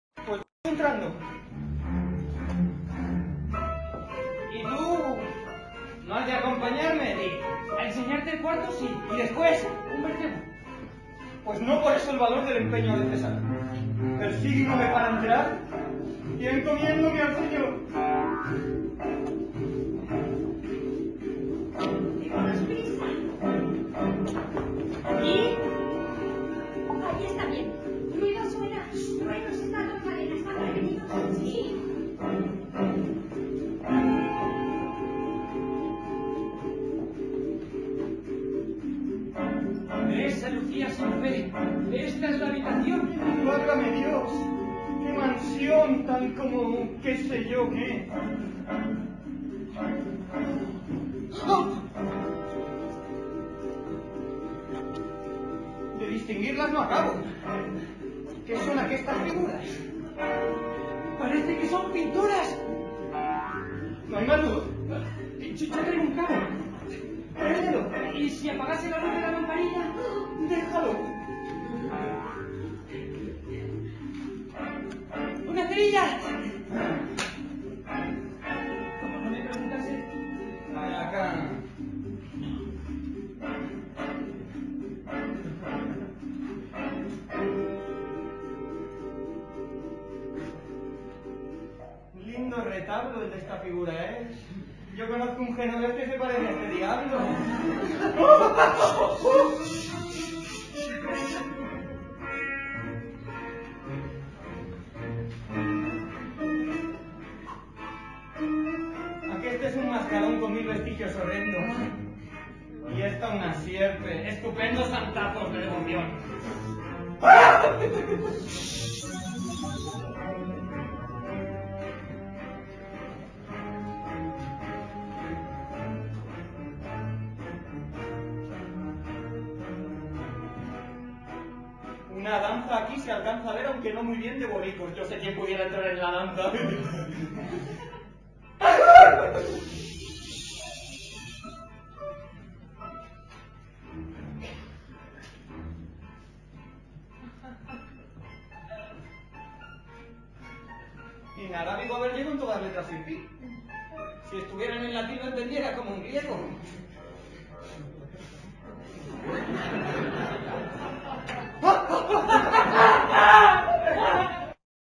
El Hechizado por fuerza [Fragmento]
Vídeo. Fragmento. El hechizado por la fuerza. Festival Internacional de Teatro Clásico de Almagro. Biblioteca Virtual Miguel de Cervantes.